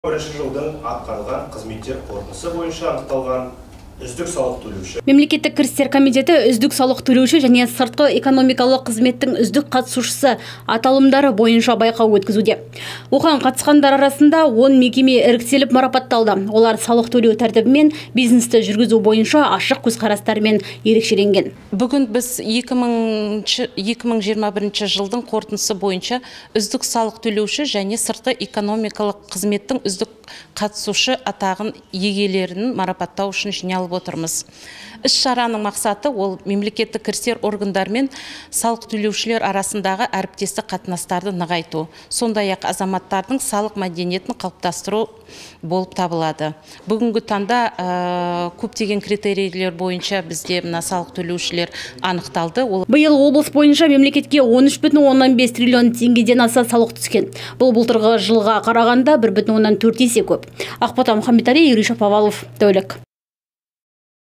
Үздік салық төлеушілер анықталды
Бүгін өңірлік кәсіпкерлер палатасы ғимаратында «Үздік салық төлеуші» және «Сыртқы экономикалық қызметтің үздік қатысушысы»  номинацияларының иелері   марапатталды.  Мемлекеттік кірістер комитеті ұйымдастырған шараның негізгі мақсаты мемлекеттік  органдар мен кәсіпкерлік субъектілер арасында  әріптестік қатынас орнату. Сонымен қатар, экономиканың түрлі саласында табысты жұмыс істейтін адал салық төлеушілерді өзгелеріне үлгі етіп, ынталандыру.Мемлекеттік кірістер комитеті  «Үздік салық төлеуші» және «Сыртқы экономикалық қызметтің үздік қатысушысы» аталымдары бойынша байқау  өткізді. Оған қатысқандар арасынан  10 мекеме іріктеліп, марапатталды. Олар салық төлеу тәртібімен, бизнесті жүргізу бойынша ашық көзқарасымен ерекшеленген. Биыл облыс бойынша мемлекетке  13,5 триллион теңгедан аса салық түскен. Бұл былтырға қарағанда 1,4 есе көп.